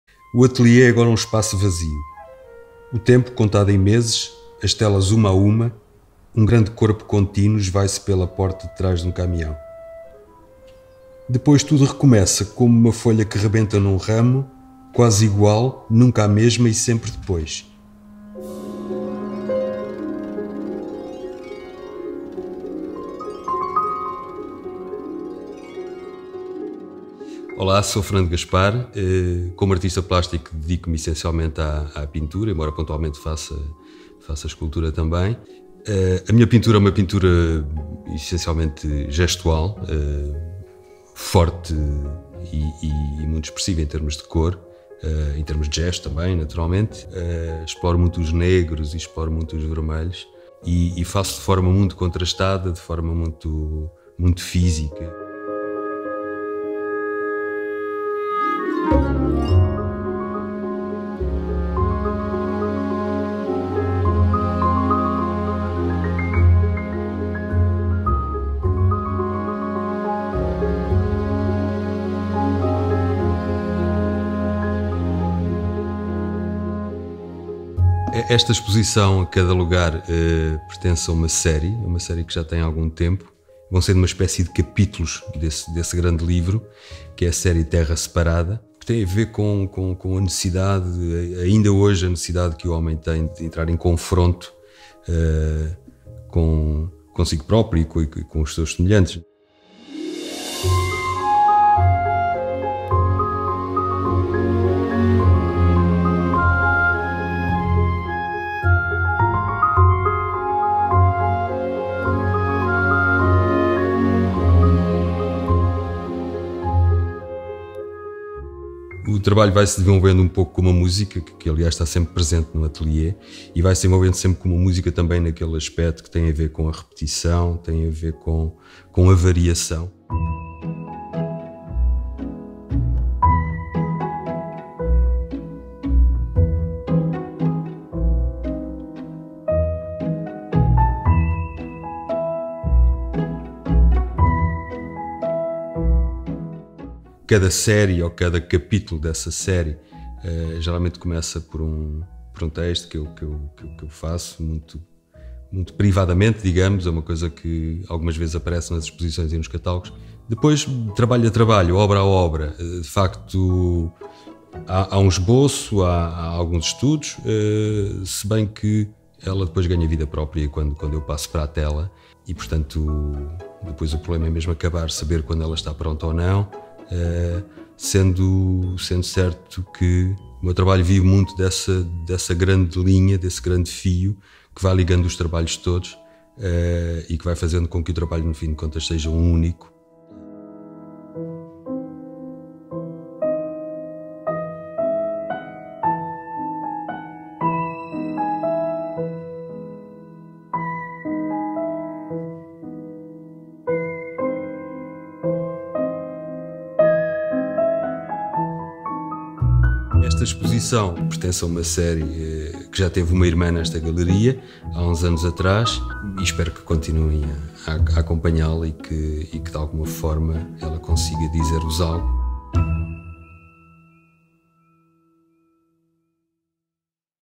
Fernando Gaspar
Fernando Gaspar is a Portuguese self-taught artist who has been exhibiting continuously since 1986, collaborating with both Portuguese and international art galleries.

Gaspar has held over sixty solo exhibitions, and has been presented with National Awards. He is represented in both public and private collections in Portugal, Spain, France, Netherlands, Belgium, United Kingdom, Denmark, Sweden, Italy, Canada, Brazil and United States.

Gaspar's practice has evolved to become increasingly reflective and unique with a total independence from current trends.